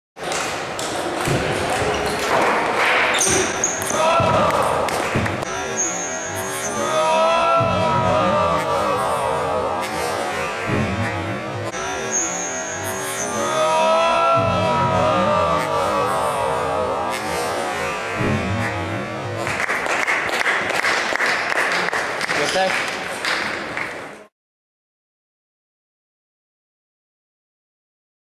My Table Tennis Drop - Will Bayley!
Will Bayley playing in the Slovenia Open 2010 against Peter Grudzian